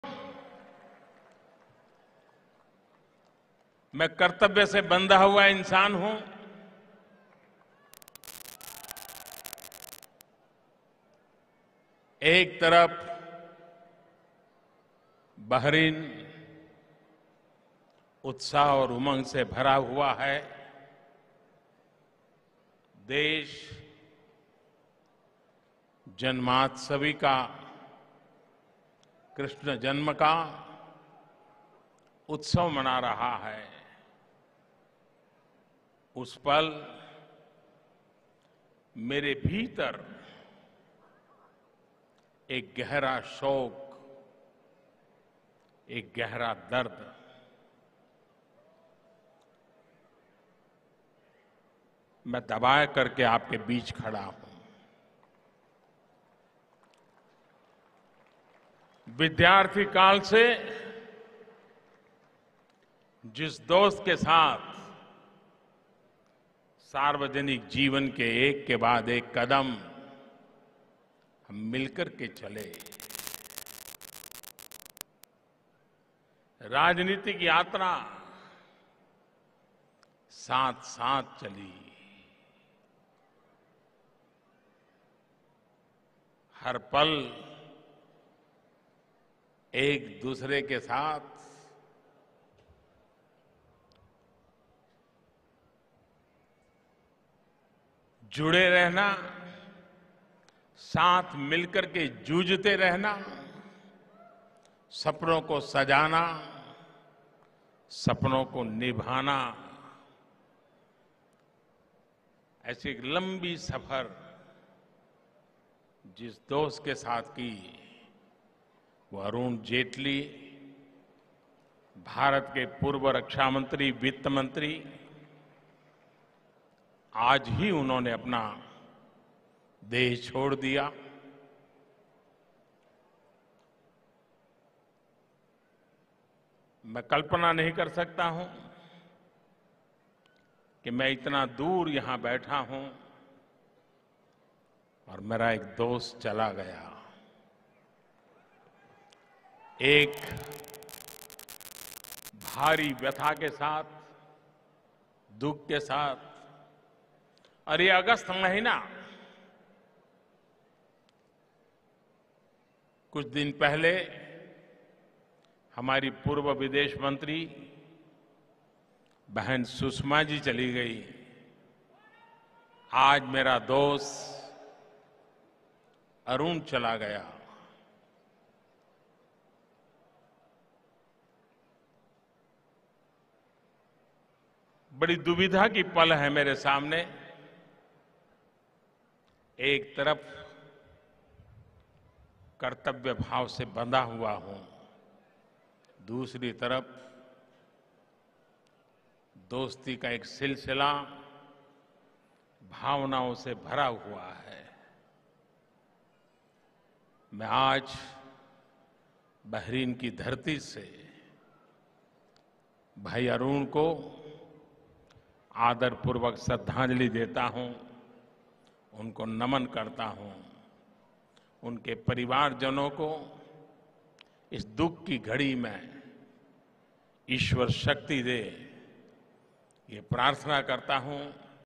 PM expresses sadness on demise of Shri Arun Jaitley
Prime Minister Narendra Modi interacts with Indian community in Bahrain